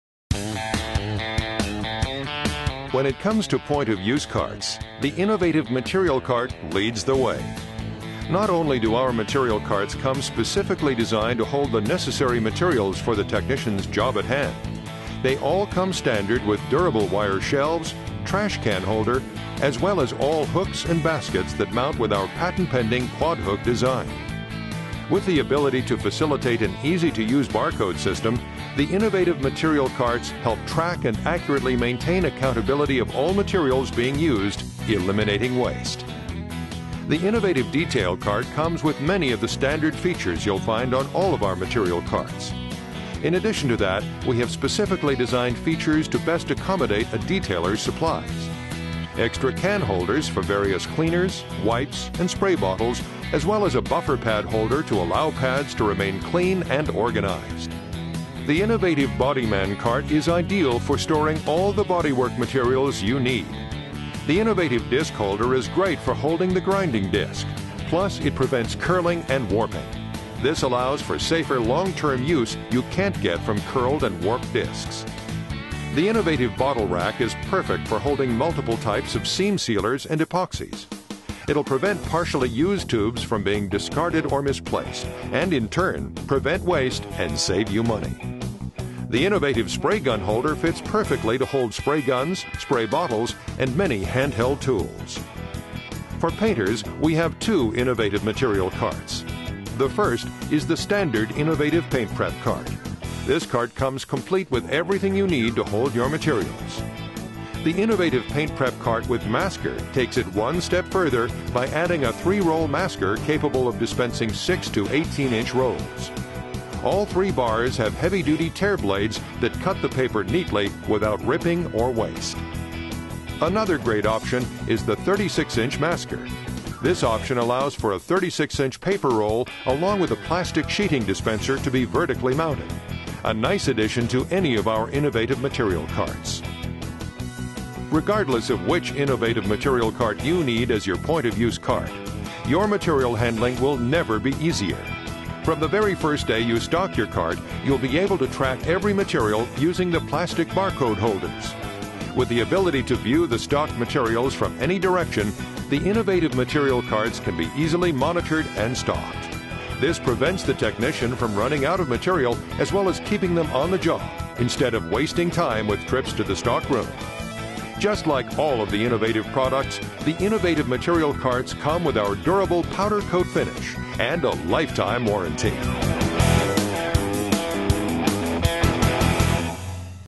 Innovative Materials Carts
This is one of a series of product videos developed for Innovative Tools for trade show, web and catalog distribution.